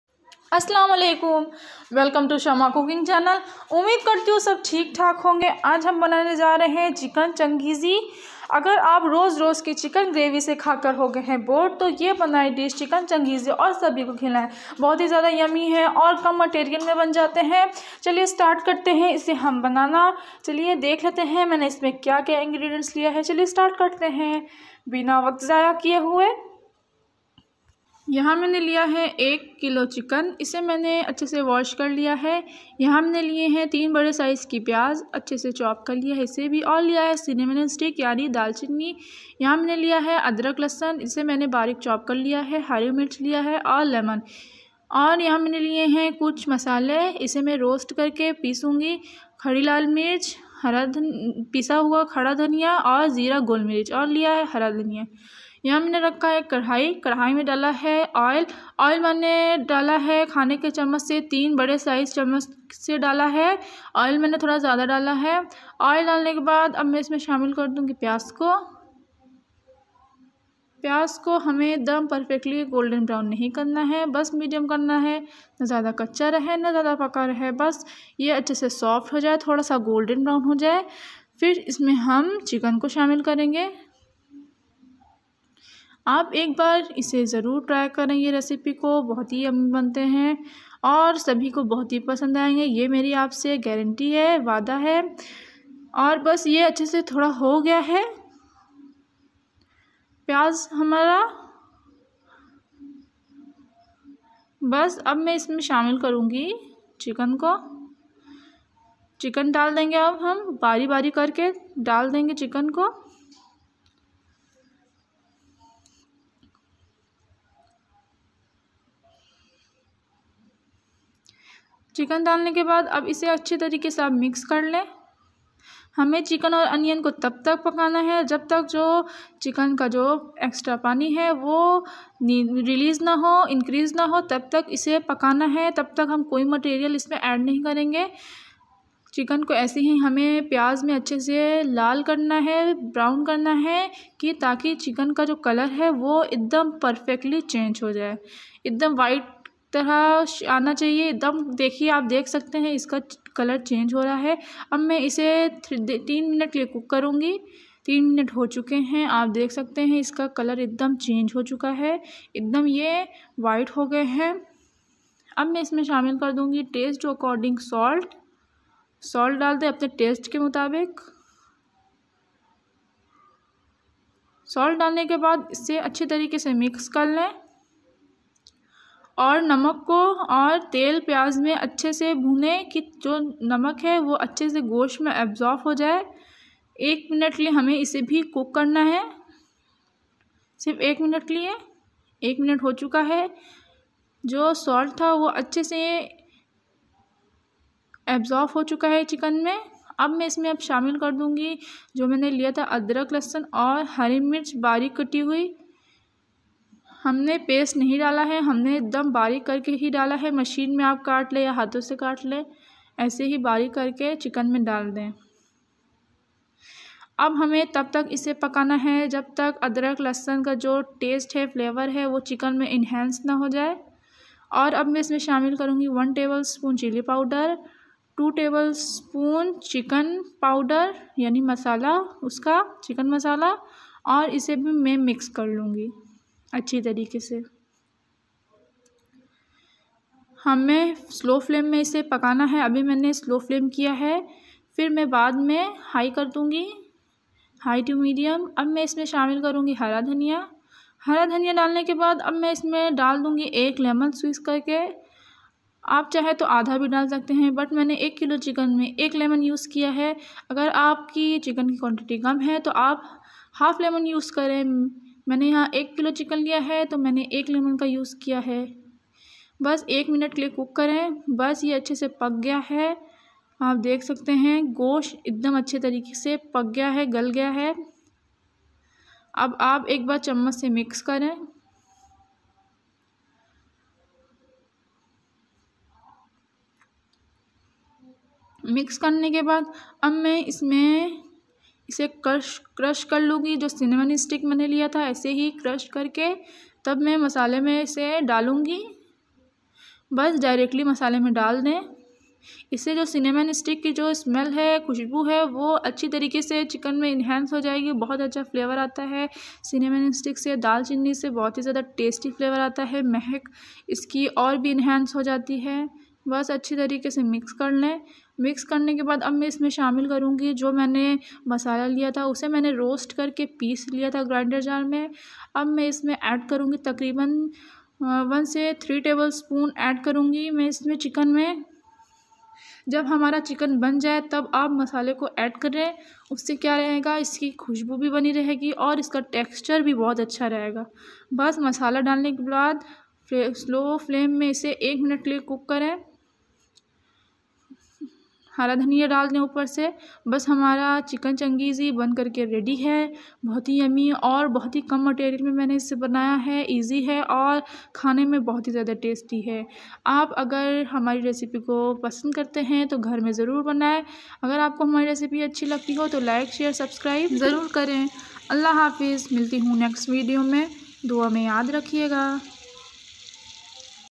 Chicken Changezi||Ak baar khay to baar baar banay||Sapicy nd tasty recipe by Shama/Cooking/Channel
Chicken Chagezi Recipe by Shama/Cooking/Channel 😘